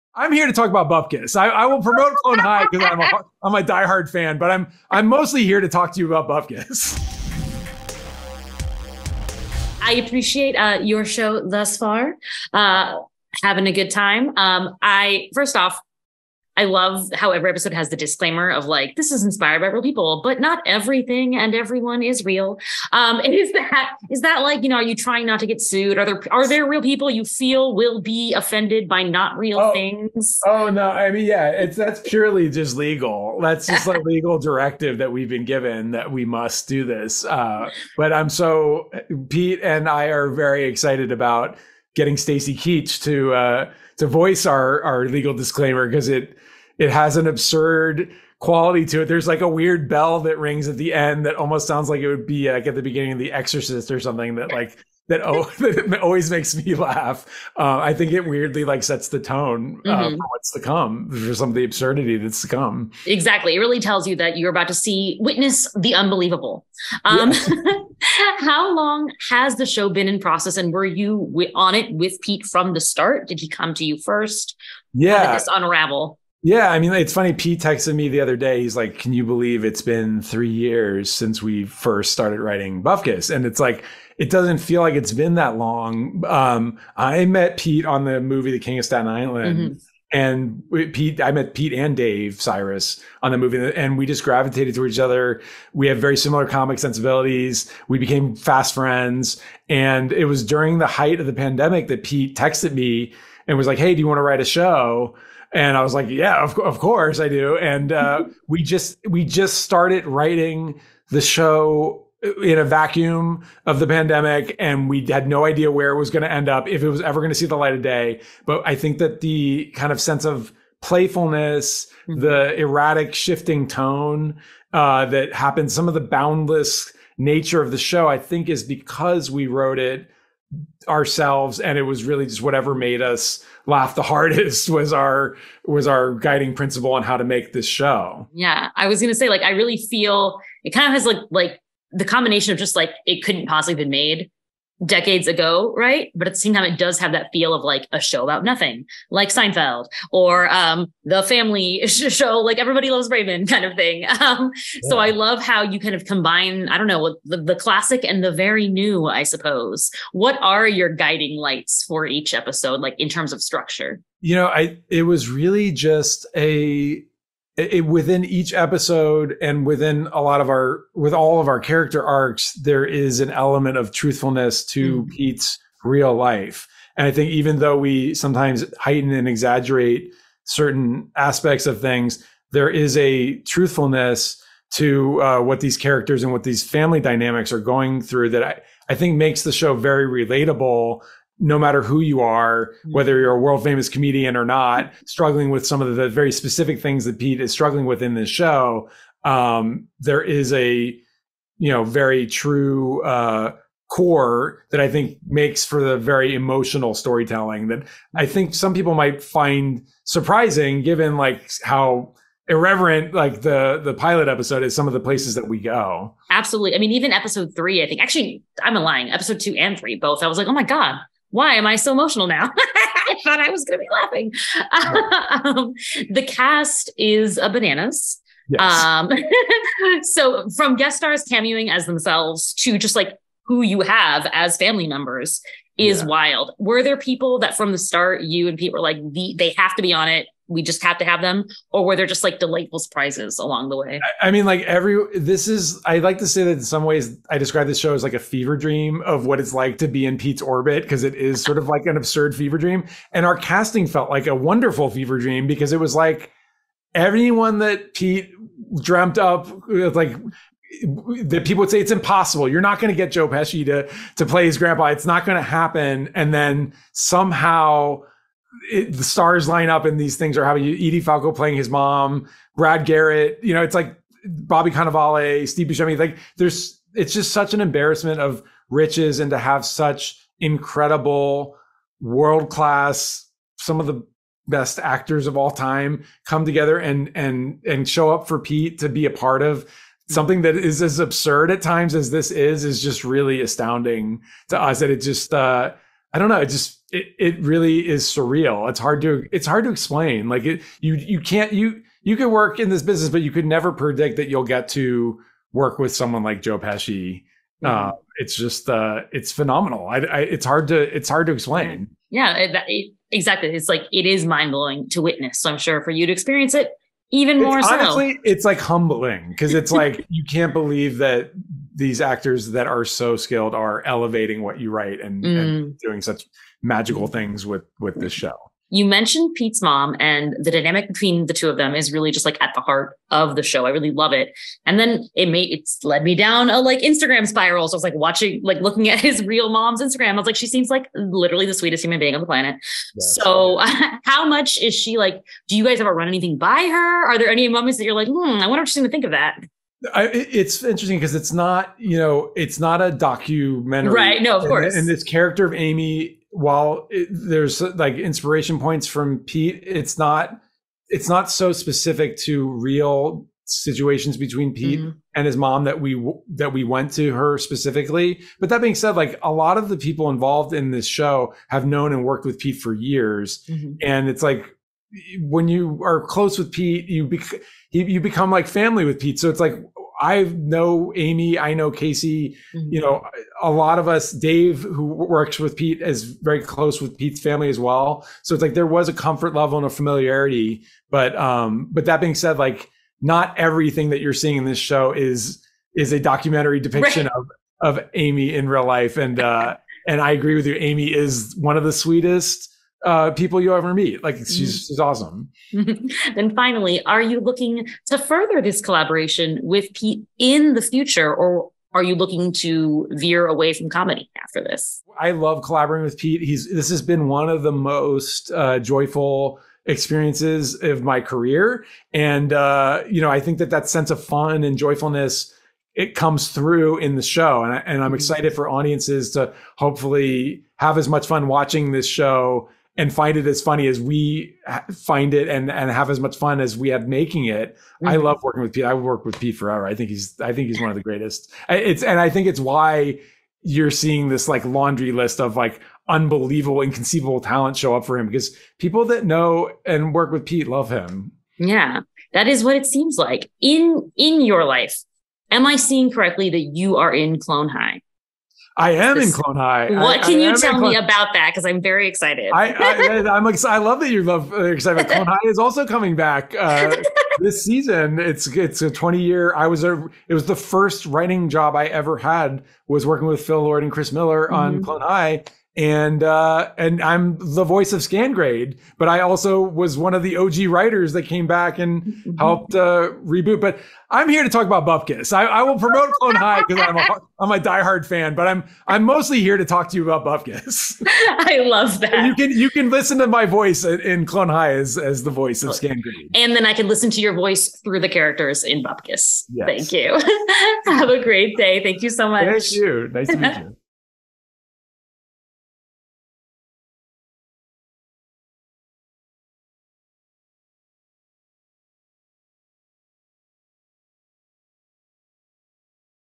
Bupkis Showrunner Judah Miller On Fictionalizing Pete Davidson's Life
Bupkis showrunner Judah Miller shares what originally inspired the Peacock comedy series, how he and Pete Davidson fictionalized the comedian's life, and why it's important to balance hilarious moments with sincerely emotional ones.

Bupkis premieres May 4 on Peacock.